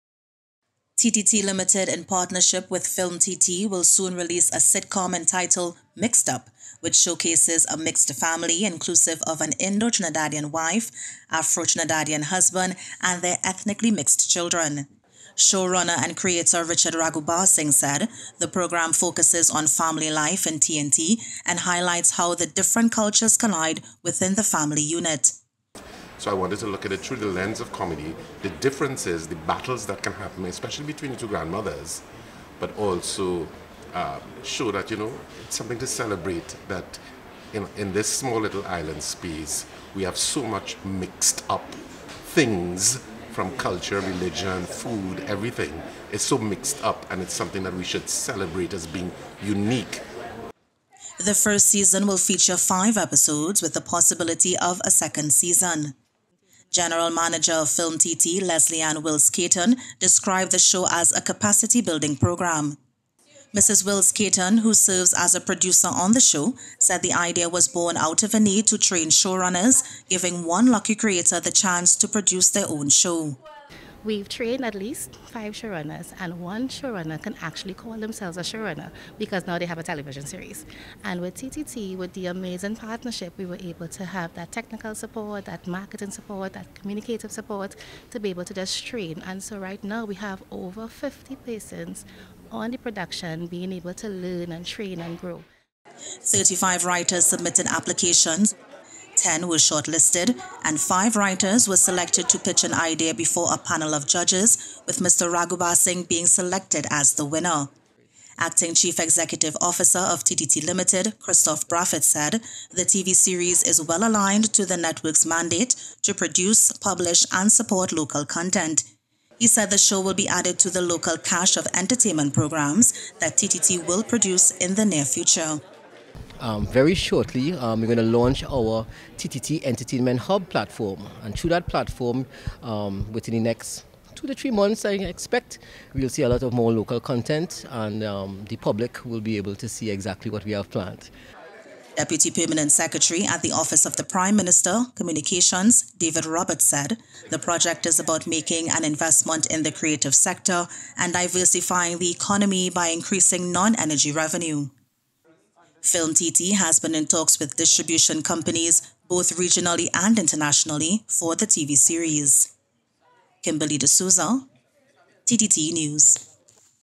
A First Look On Set Of "Mixed Up"
"Culture and diversity collide in TTT and Film TT's upcoming sitcom "Mixed Up", and TTT News got a first-hand look at the set and TV show on Monday.
MIXED UP, conceptualised by Film TT, is the On Set capacity development project.
The TV series is being regarded as the first local television sitcom to be produced in a studio-like setting."